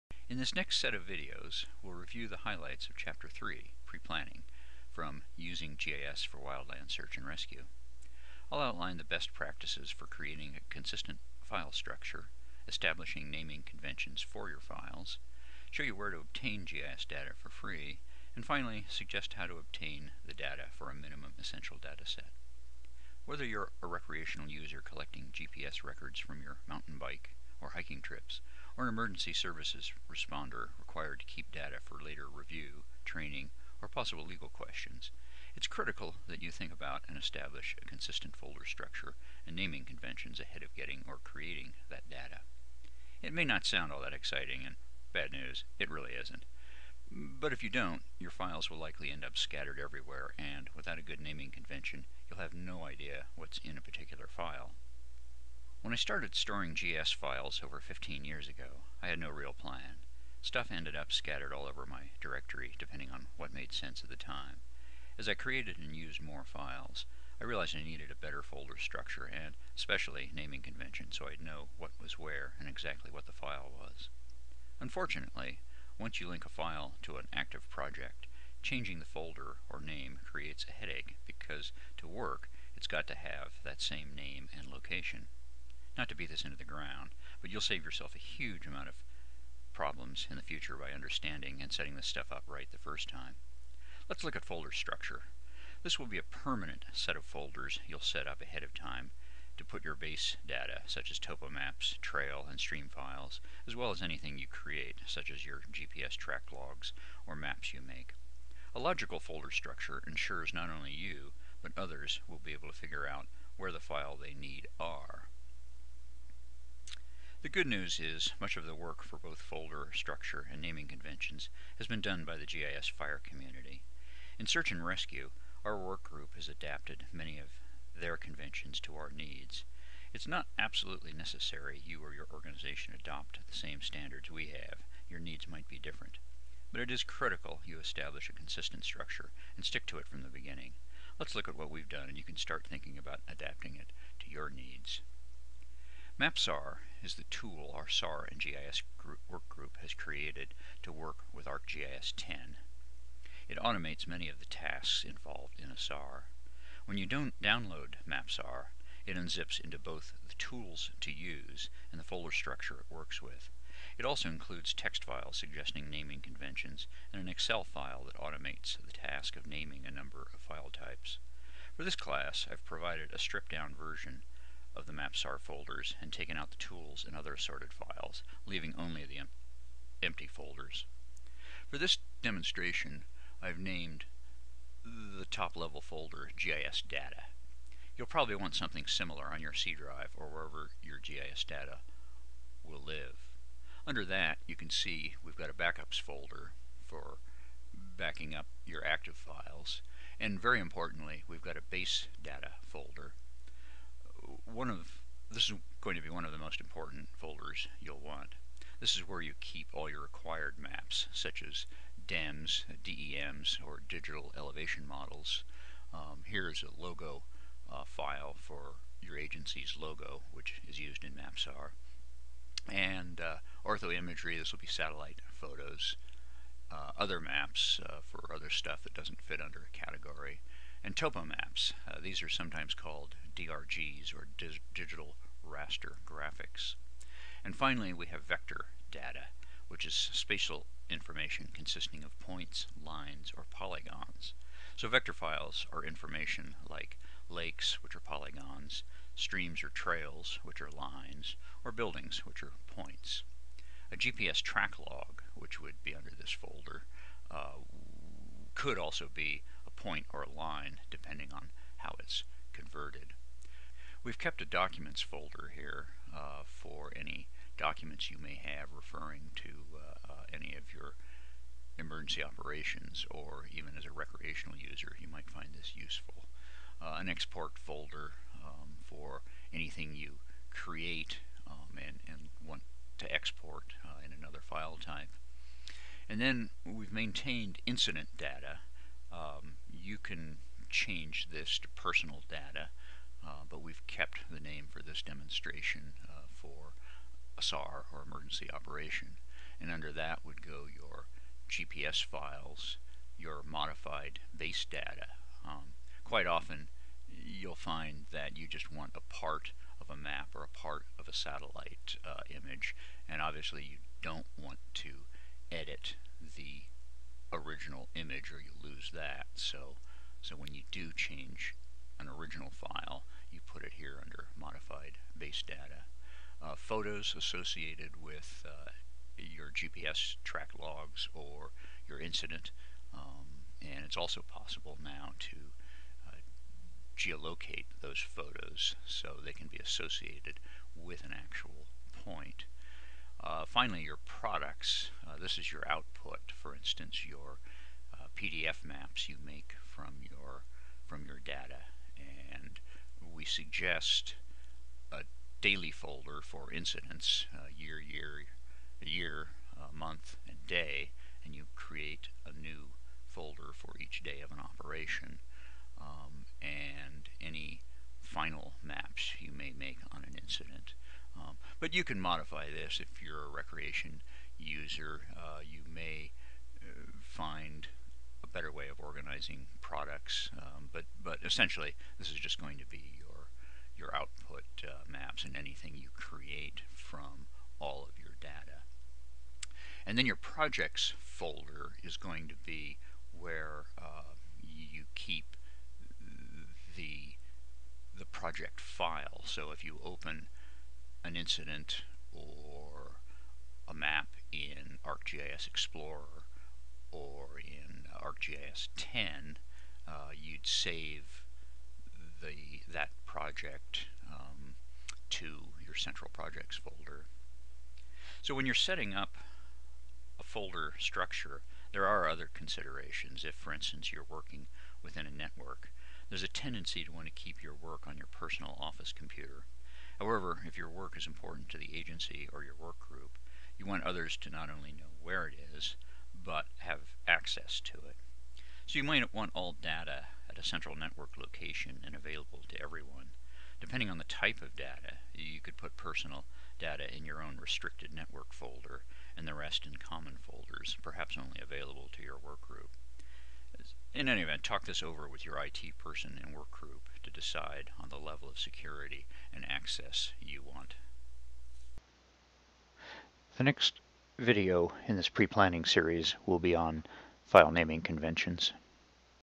03a (HD) GIS -- Preplanning: Creating a Folder Structure in GIS
Creating a consistent and logical file structure. Columbia College GIS
classes from Chapter 3: Preplanning, Using GIS for Wildland Search and
Rescue (free at mapsar.net).